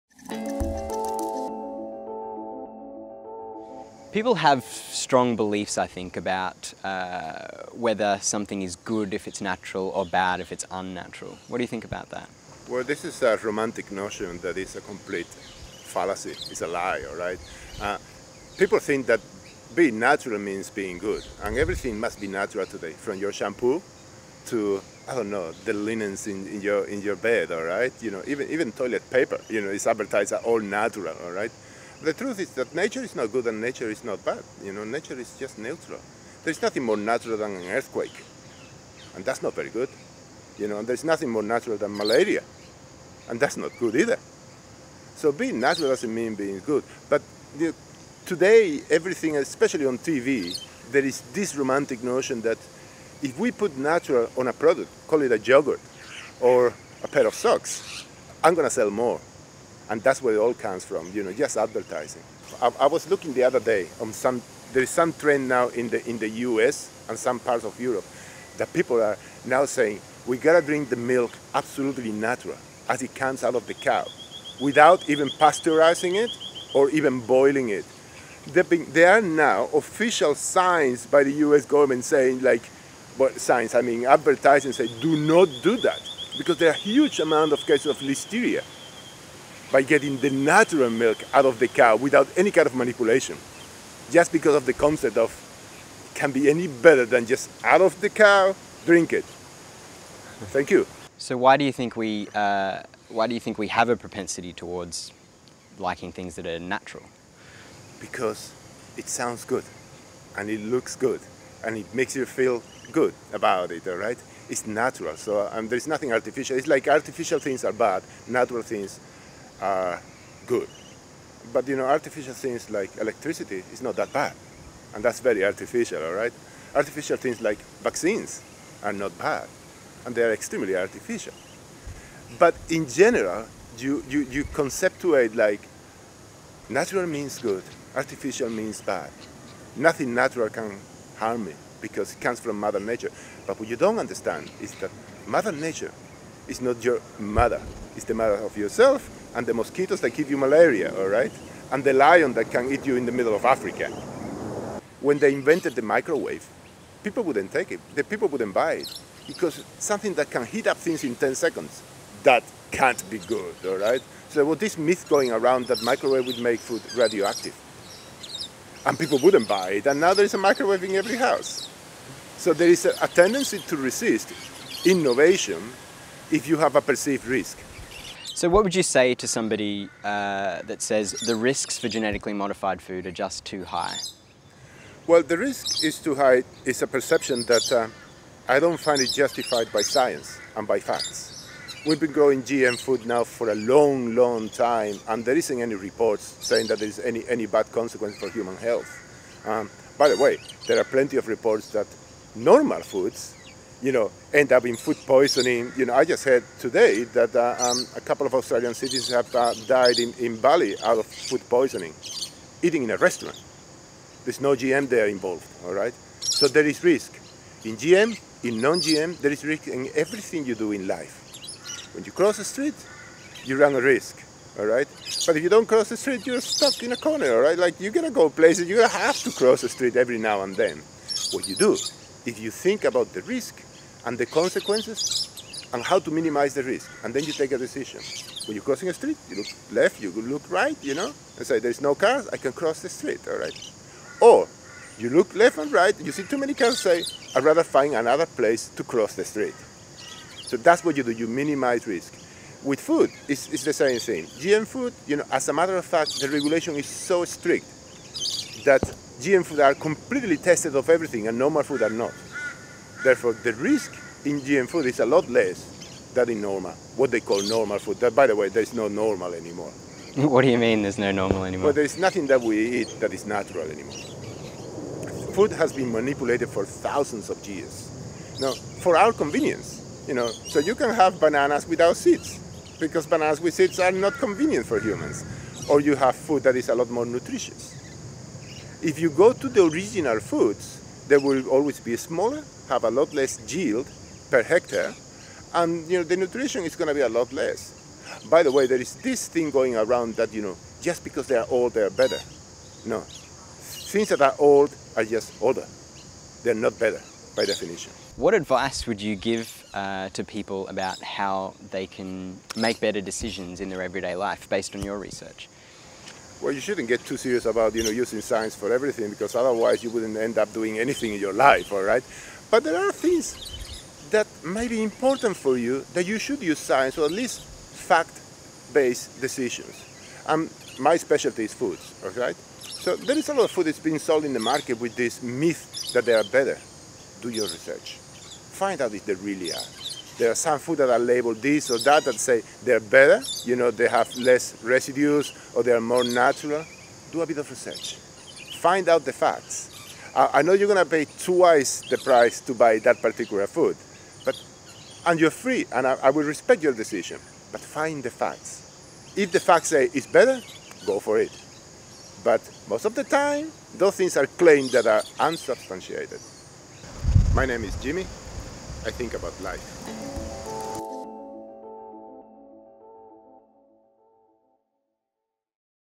Episode 9 − Health Claims: Conversation with Jimmy Botella
We've seen that what we perceive and remember is literally shaped by our experiences and expectations. The placebo effect is another example of this phenomenon. Symptoms improve with treatments that have no active ingredients. We introduce several reasons for this effect along with beliefs that "natural" is better, and perceived risks of vaccination.